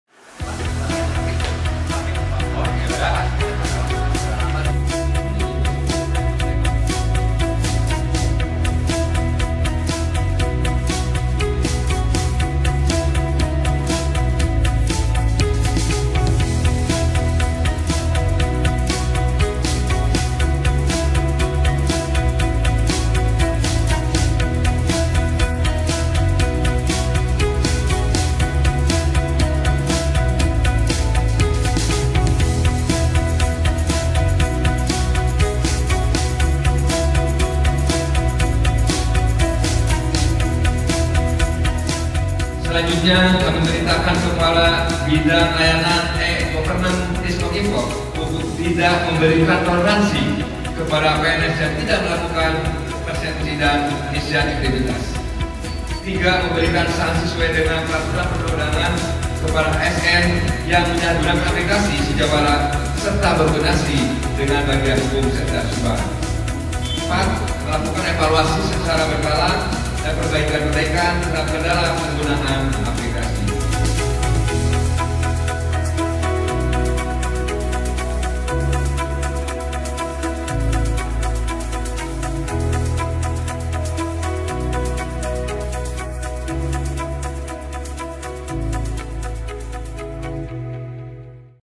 Sekda Subang Membuka Bimtek Sosialisasi Aplikasi Si JAWARA
Sekretaris Daerah Kabupaten Subang H. Asep Nuroni, S. Sos., M. Si., membuka Bimtek Sosialisasi Aplikasi Si JAWARA (Sistem Kinerja Pegawai dan Remunerasi) yang dilaksanakan di Laska Hotel Subang, Selasa (06/02/24).